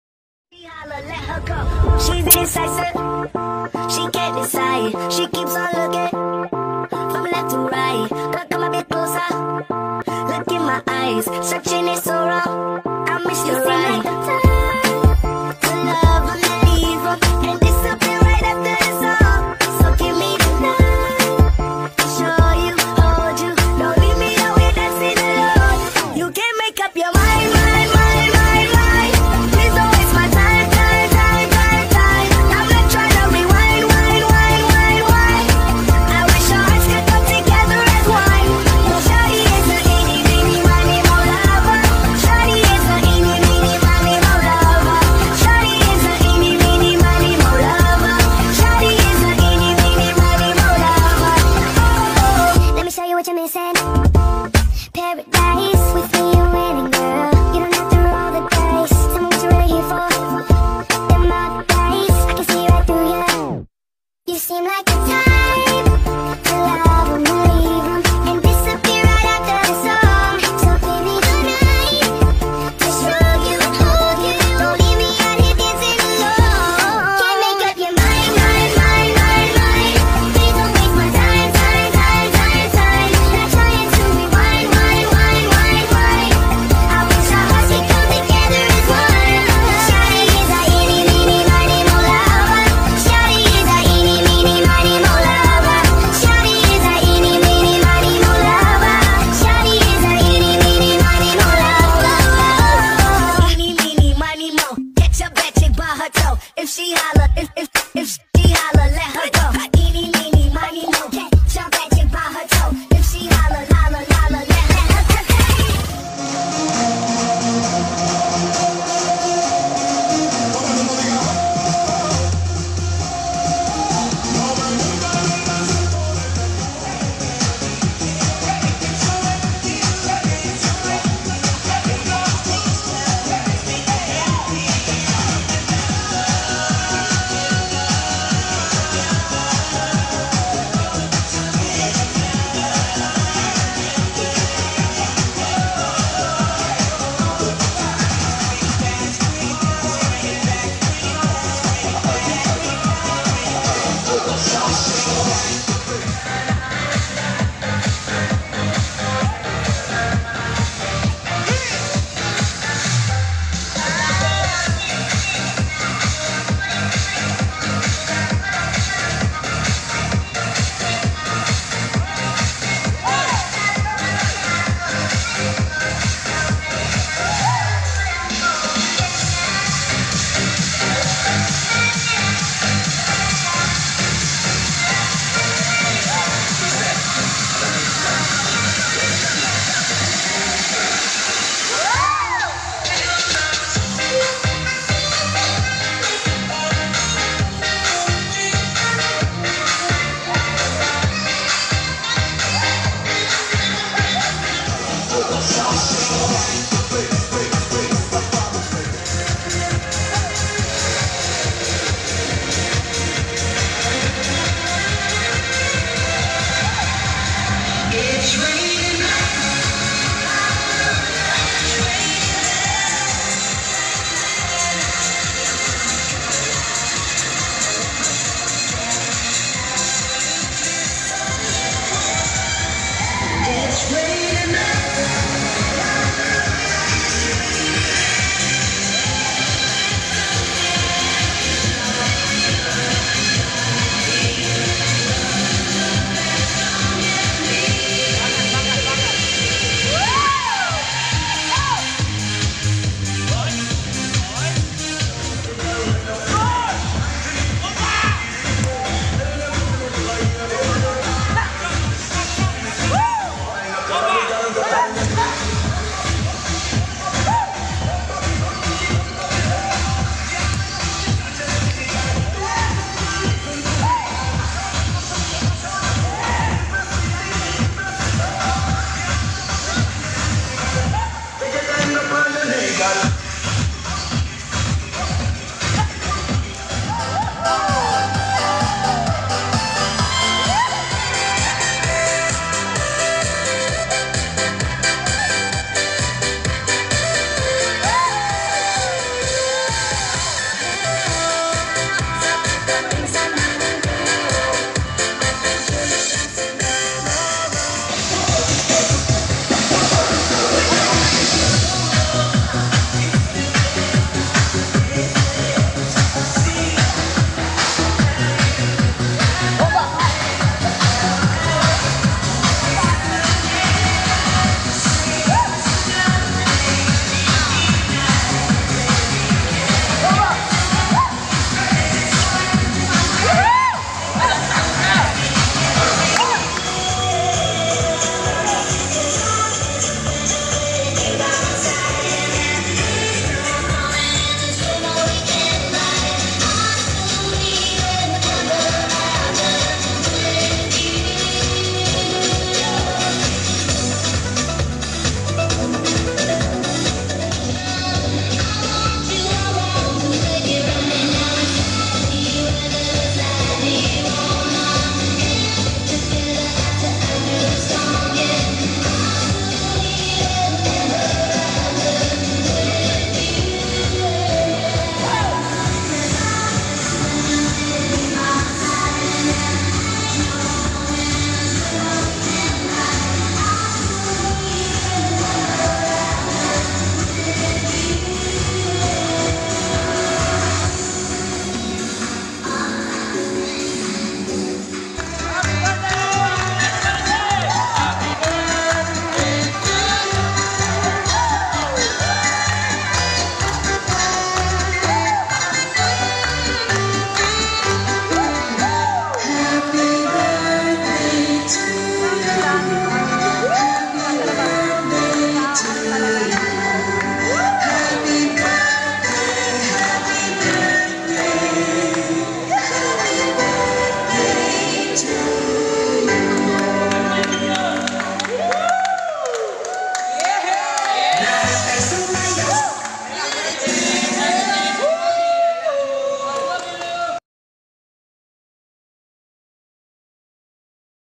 ZUMBA SUCAT DANCE FITNESS AT GREENHEIGHT VILLAGE WITH JAYVIE AND SOUTHVIBES
THIS IS WHAT HAPPEN WHEN YOU MISS THEM. IT'S A SURPRIZE!!! DINAYO NAMIN SILA SA KANILANG MINI EVENT. THANKS